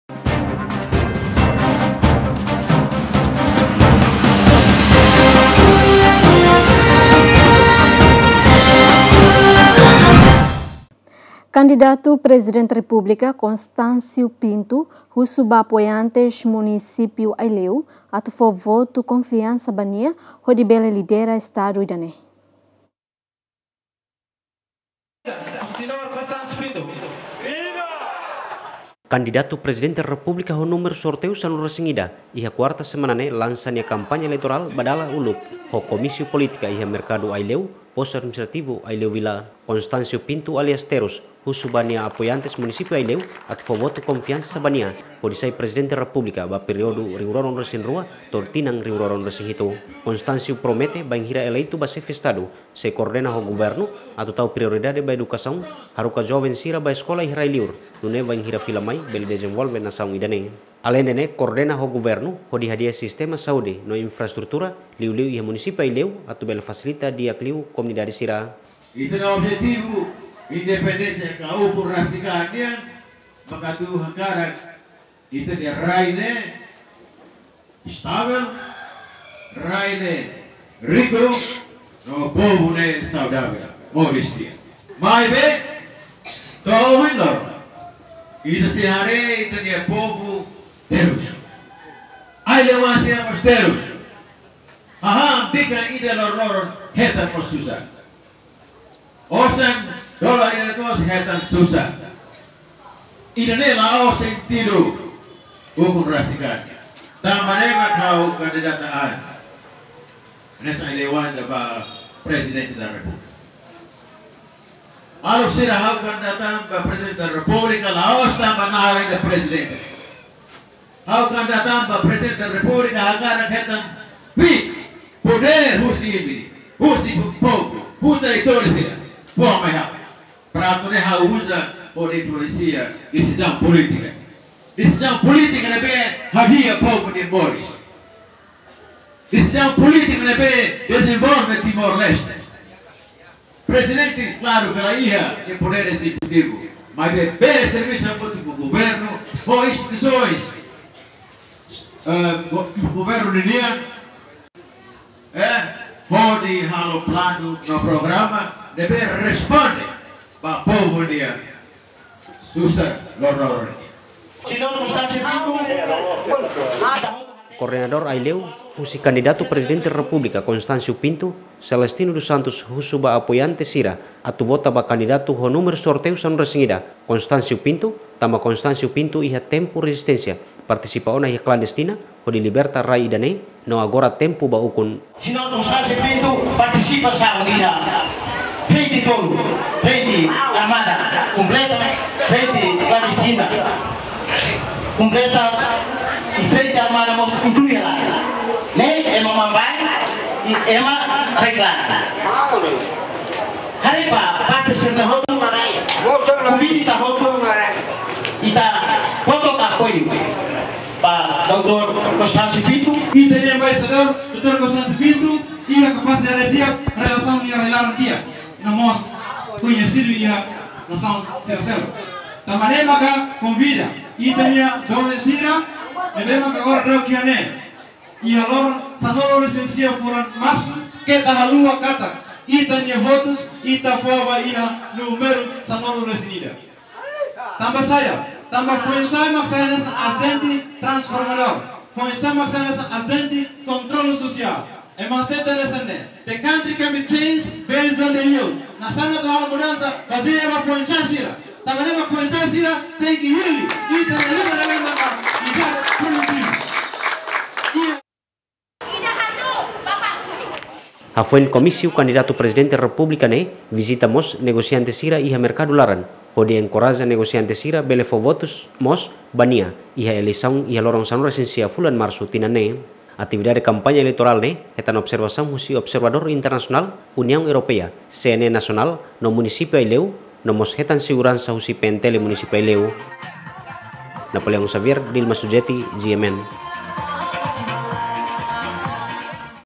Kandidatu PR "Constancio Pinto" husu apoiantes Munisípiu Aileu atu fo votus konfiansa ba nia
Kandidatu PR Constancio Pinto husu ba apoiantes Munisípiu Aileu atu fo votus konfiansa ba nia hodi lidera Estadu ida ne’e